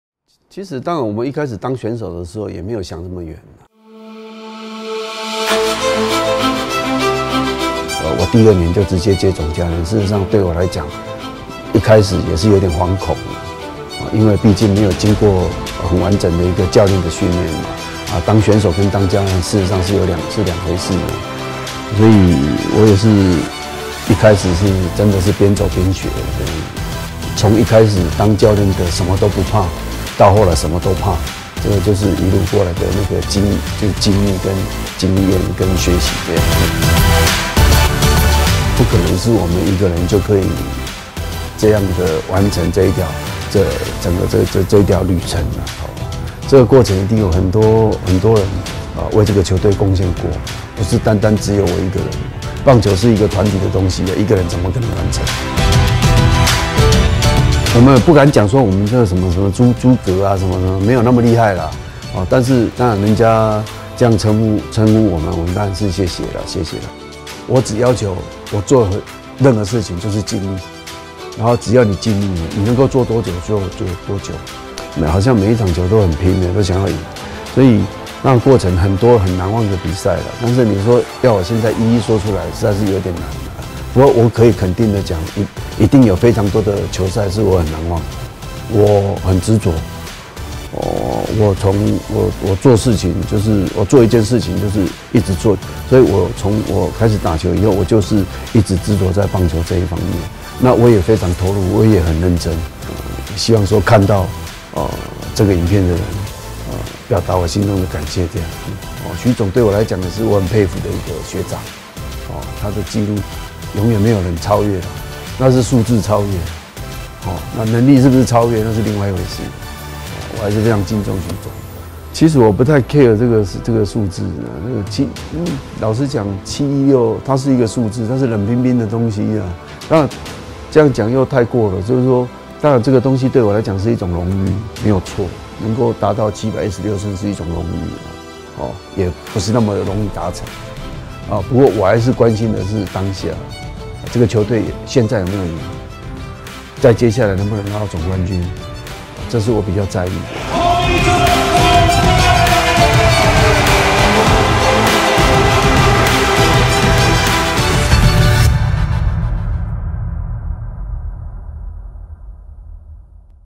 【洪總執著的棒球路】
恭喜洪一中總教練成為中華職棒最多勝的總教練

執著的棒球路～洪總史上最多勝特輯（搶先慶祝版）與10號隊友第一時間分享洪總的榮耀！

完整版將在7月22日主場首播，見證洪總榮耀的每一刻！